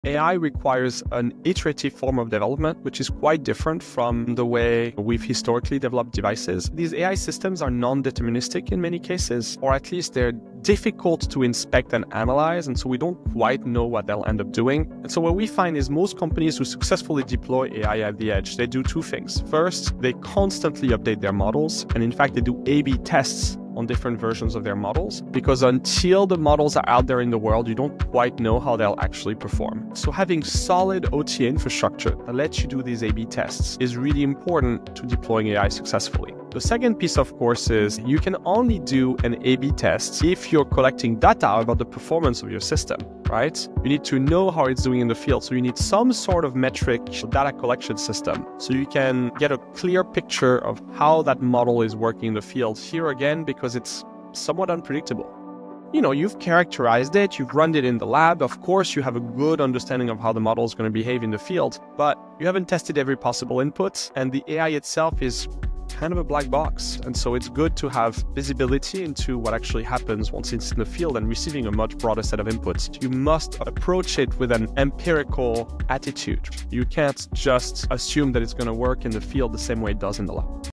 AI in Embedded Systems: A Black Box You Must Learn To Control
AI isn’t predictable, it adapts, making embedded engineering even more complex. A model that works in the lab might fail in the real world. So, how do successful teams deploy AI at the edge?

A/B test models in the field—controlled environments aren't enough.

Collect real-world performance data—observability tools are key.
AI deployment isn’t a one-and-done process. It requires constant iteration and real-world validation.

What would make AI adoption easier for your team?